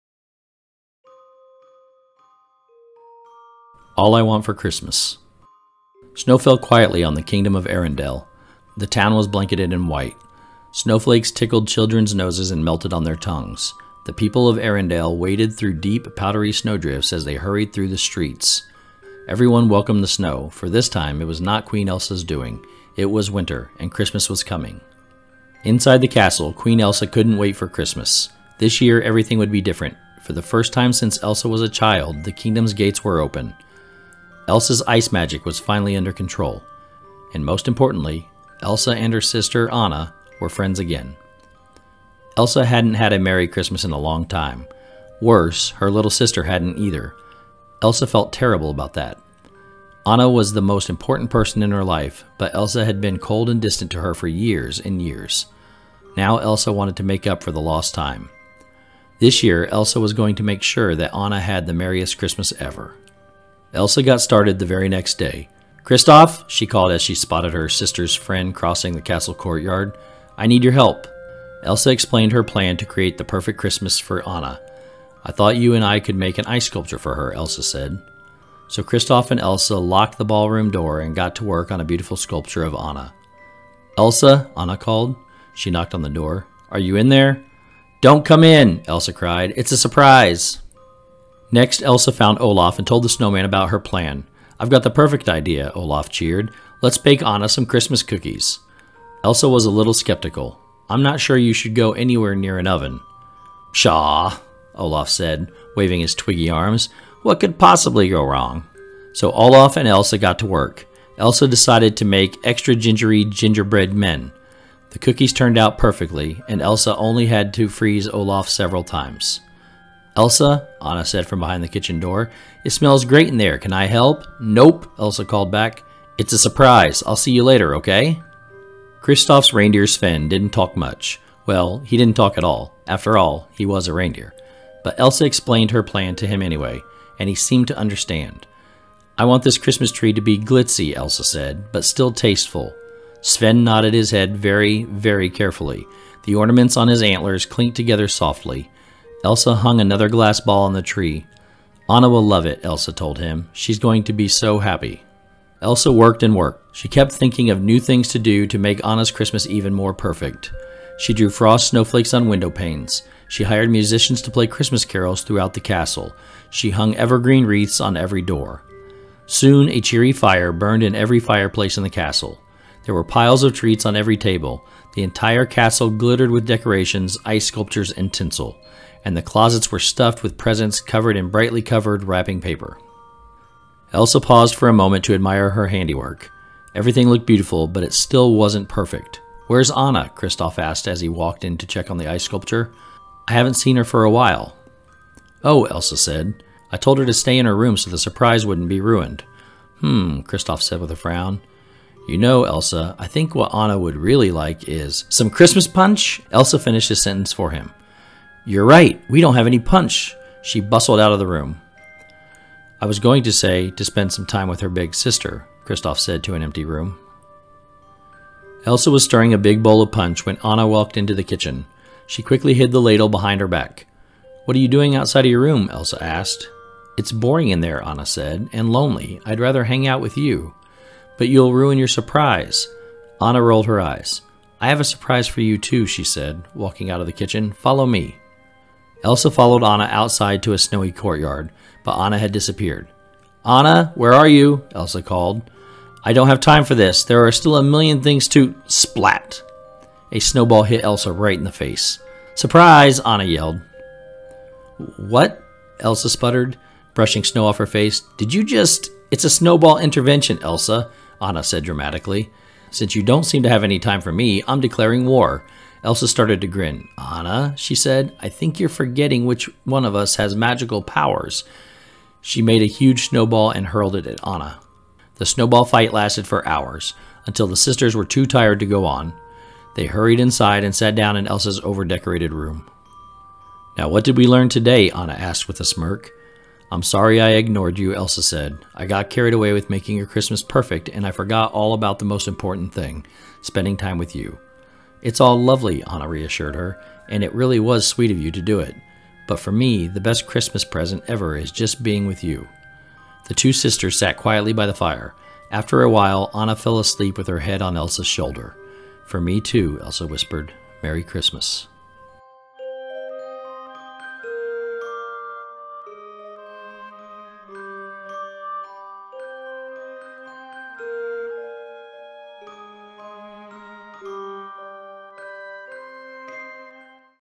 Vlogmas Day 19 | All I Want For Christmas
This is a difficult task and I encourage you to check out other creators for the work they have put in. This is my third Vlogmas since I joined YouTube and I have themed the previous 2. This year my intentions are to read Christmas stories with illustrations. Today's Vlogmas reading is All I Want For Christmas featuring characters from Disney's Frozen!